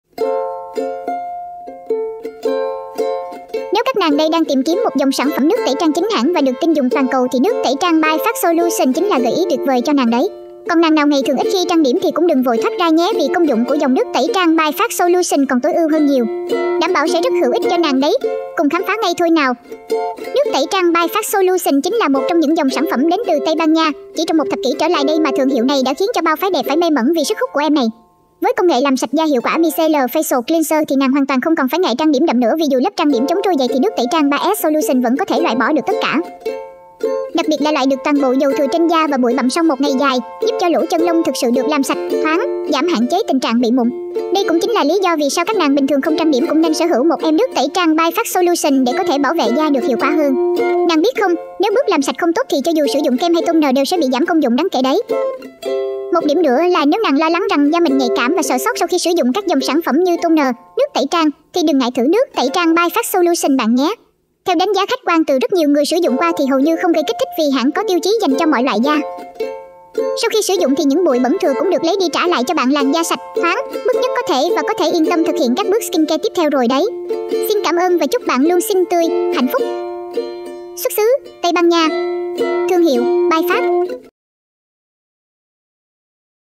Nước tẩy trang Byphasse Solution dành cho mọi loại da 500ml
Nếu các nàng đây đang tìm kiếm một dòng sản phẩm nước tẩy trang chính hãng và được tin dùng toàn cầu thì Nước tẩy trang Byphasse Solution chính là gợi ý tuyệt vời cho nàng đấy. Còn nàng nào ngày thường ít khi trang điểm thì cũng đừng vội thoát ra nhé vì công dụng của dòng Nước tẩy trang Byphasse Solution còn tối ưu hơn nhiều. Đảm bảo sẽ rất hữu ích cho nàng đấy. Cùng khám phá ngay thôi nào